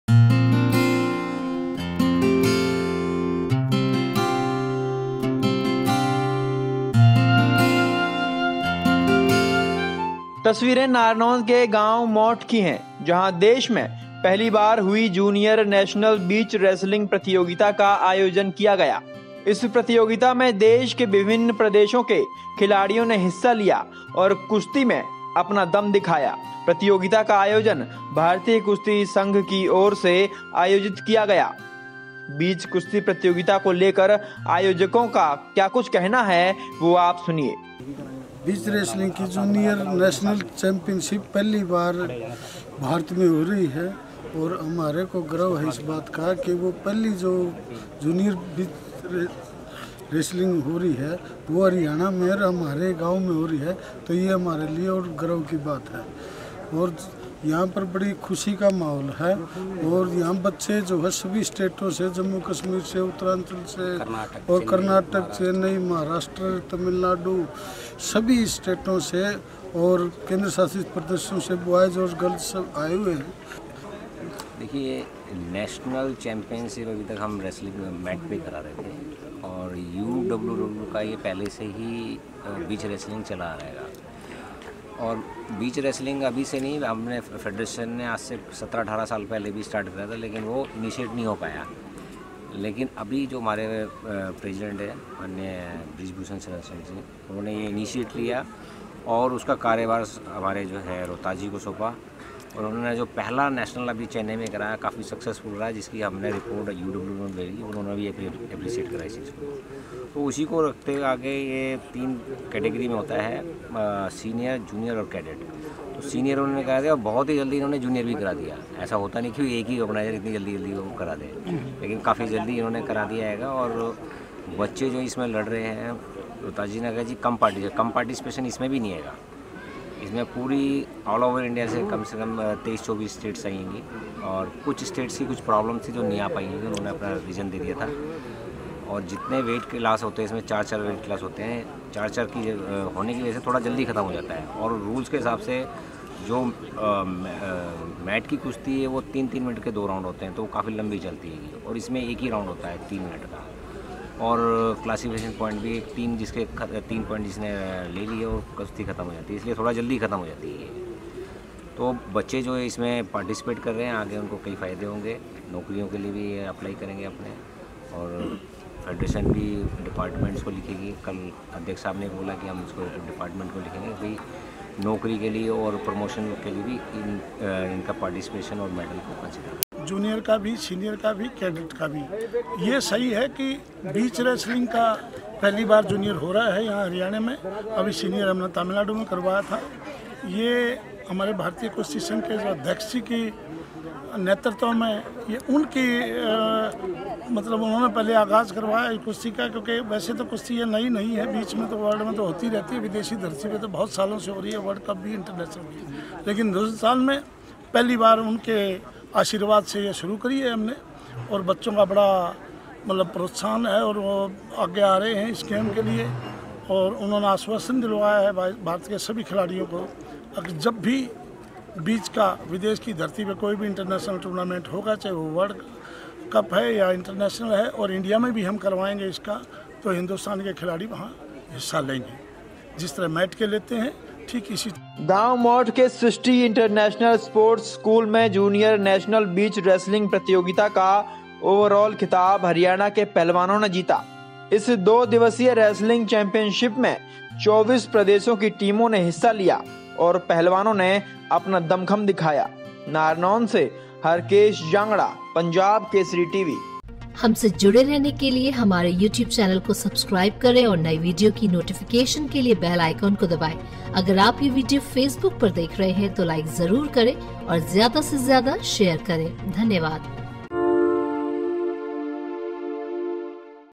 गांव मोठ में जूनियर नेशनल बीच रेसलिंग चैंपियनशिप का हुआ आयोजन
तस्वीरें नारनौंद के गांव मोठ की है जहां देश में पहली बार हुई जूनियर नेशनल बीच रेसलिंग प्रतियोगिता  का आयोजन किया गया...इस प्रतियोगिता में देश के विभिन्न प्रदेशों के खिलाड़ियों ने हिस्सा लिया और कुश्ती में अपना दम दिखाया,,, प्रतियोगिता का आयोजन भारतीय कुश्ती संघ की और से आयोजित किया जा रहा है..बीच कुश्ती प्रतियोगिता को लेकर आयोजकों का क्या कुछ कहना है वो आप पहले सुनिए...